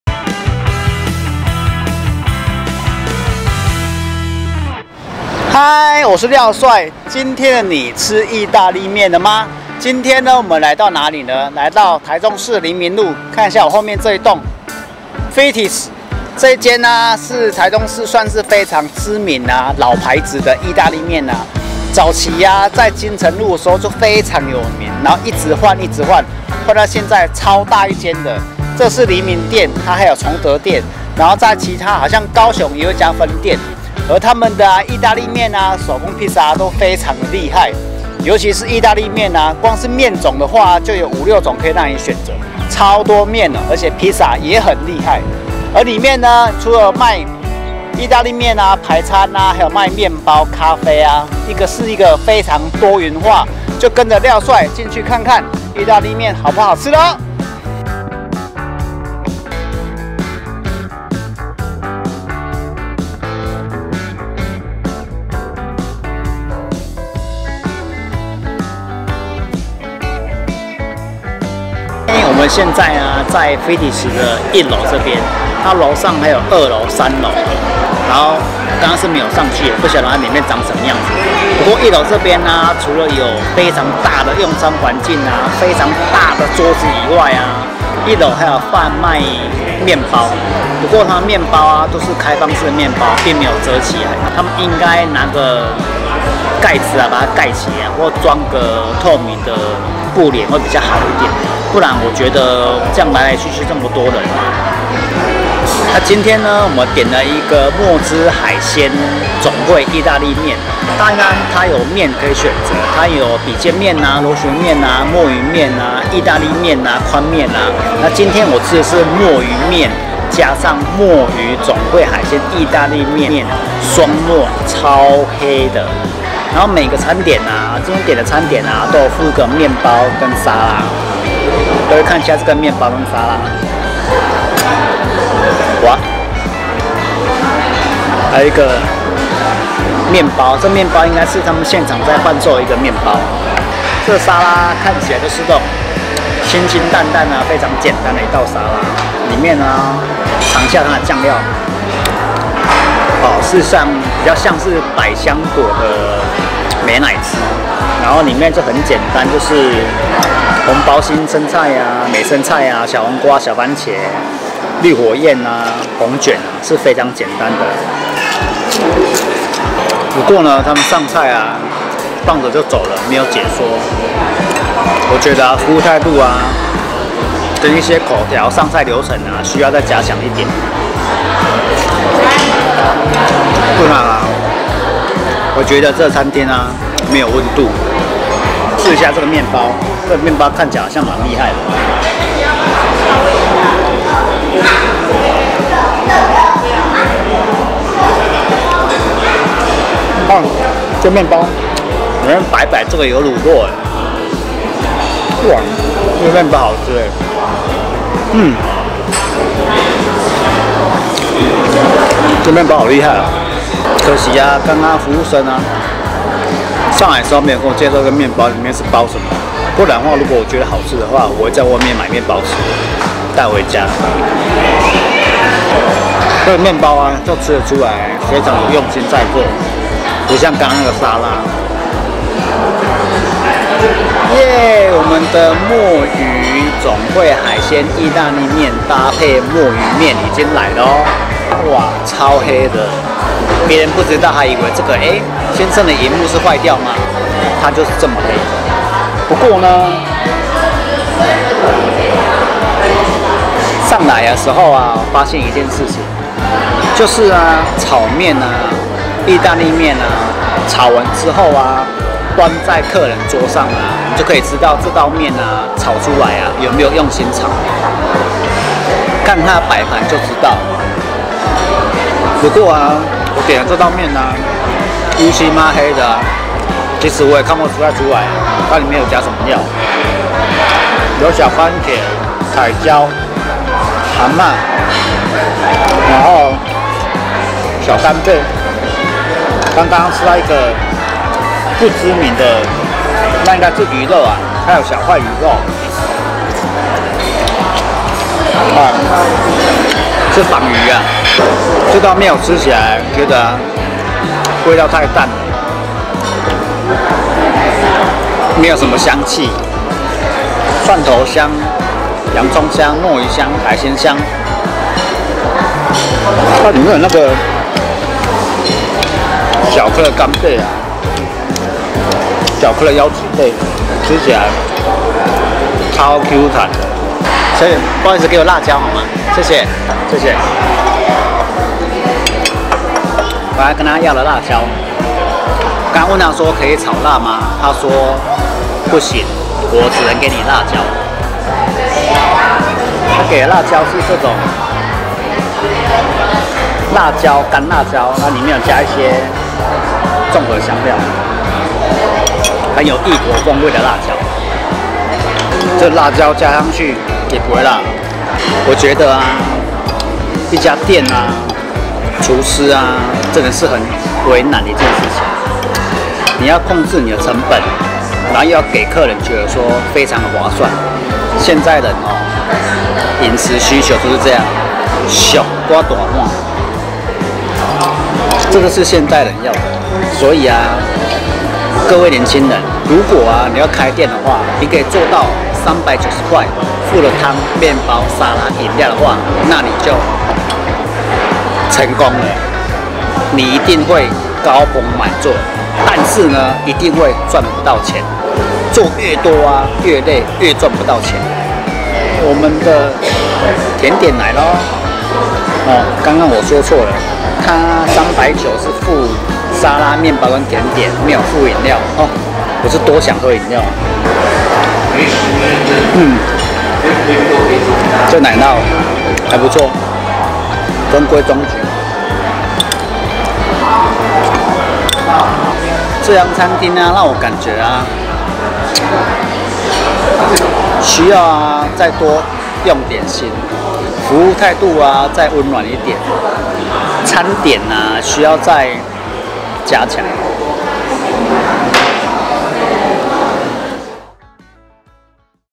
fatty's~吃義大利麵有各式手工麵可以選擇，超誇張。連Pizza的口味多到眼花暸亂。
fatty's~吃義大利麵有各式手工麵可以選擇，超誇張。連Pizza的口味多到眼花暸亂。不過這次的味道差了一點，服務態度，上菜口條要再加強。